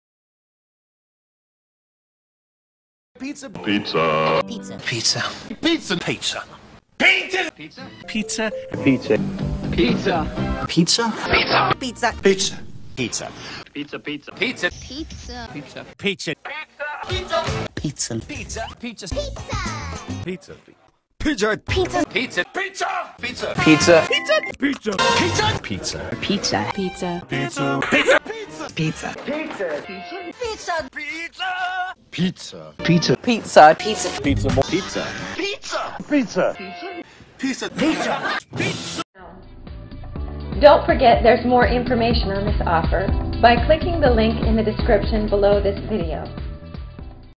Pizza Delivery Near Me Augusta GA - 60 Utterances of Pizza in Movies - Hungry Howies Augusta GA
A funny look at 60 Utterances of Pizza in Movies.